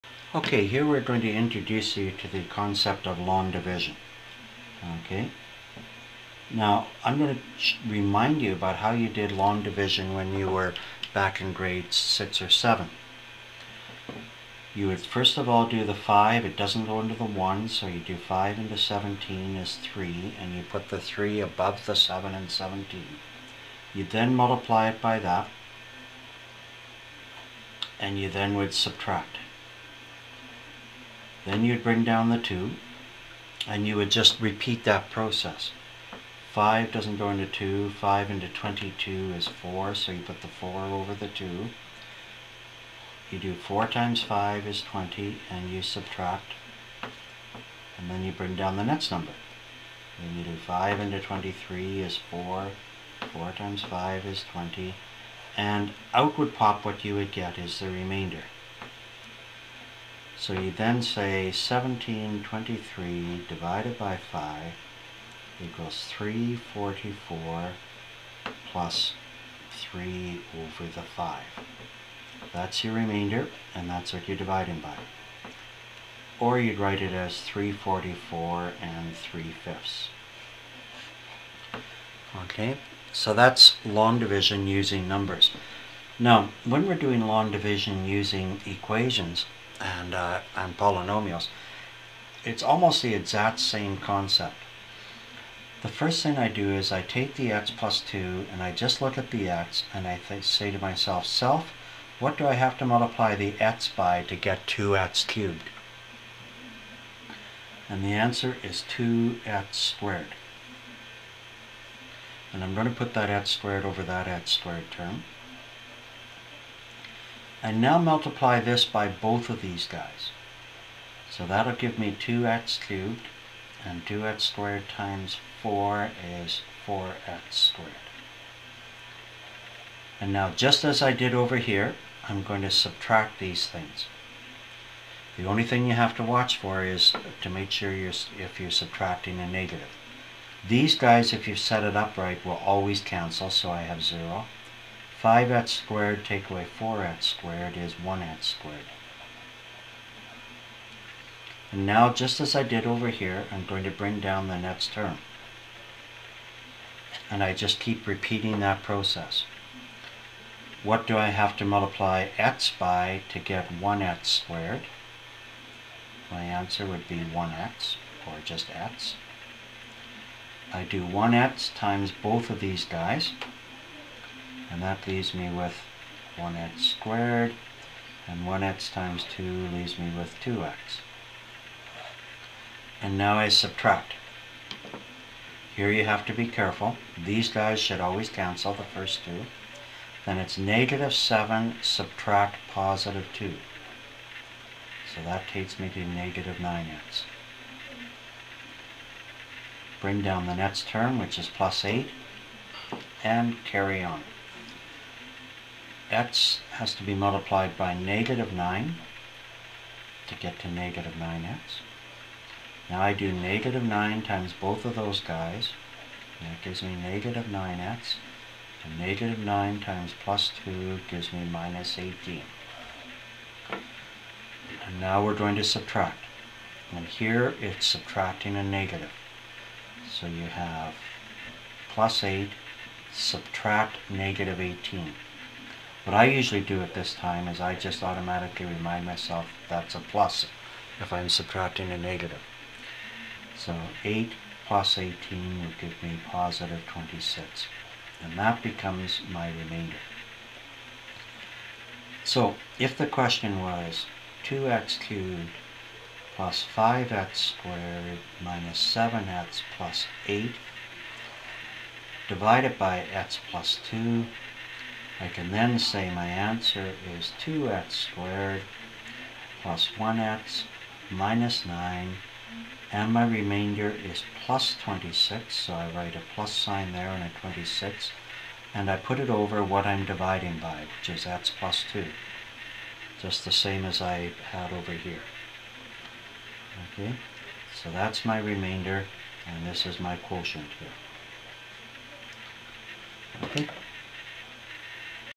Long Division in algebra.
How to divide a polynomial by a binomial using long dividsion. This is video 25 of 51 in the solving equation series. The next three series are "Word Problems", "Factoring" and "Graphing".